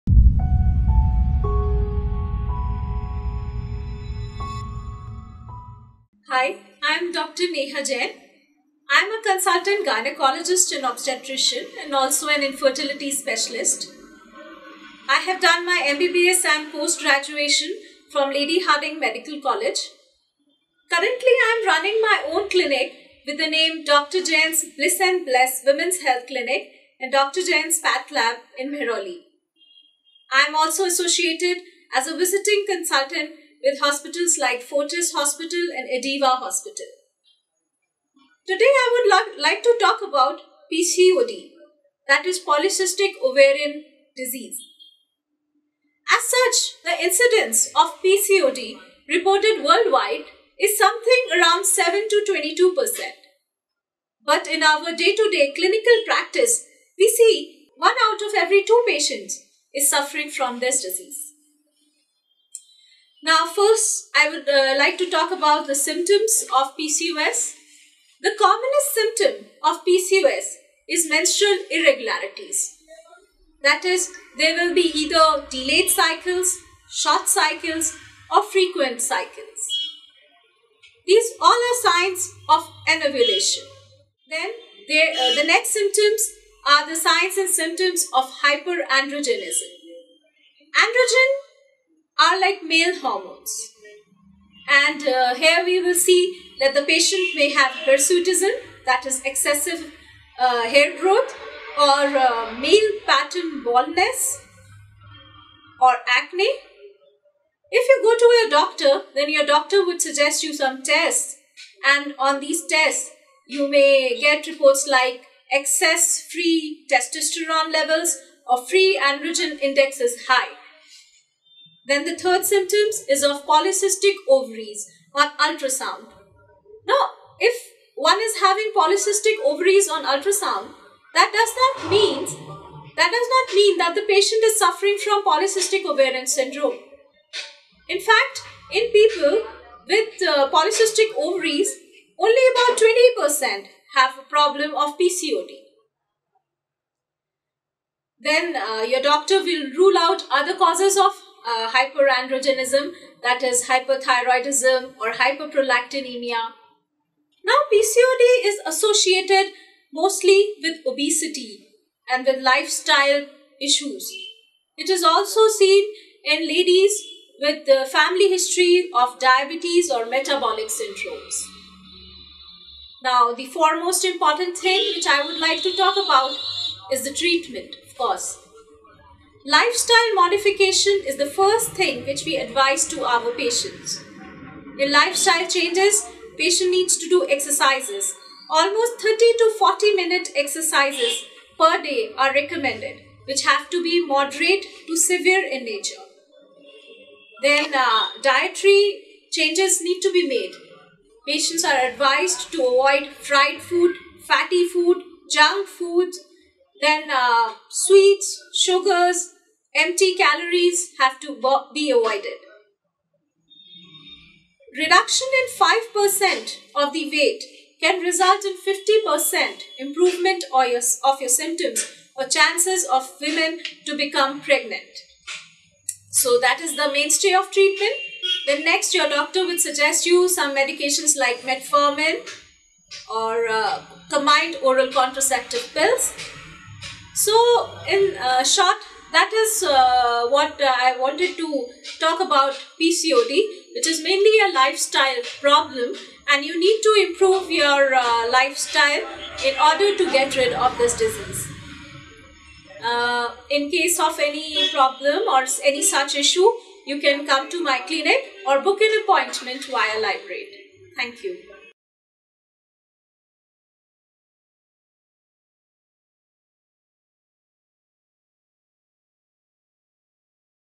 Lybrate / Dr Neha Jain Talks About PCOS
Polycystic ovary syndrome (PCOS) is a hormonal disorder which is most common among the reproductive age group of women.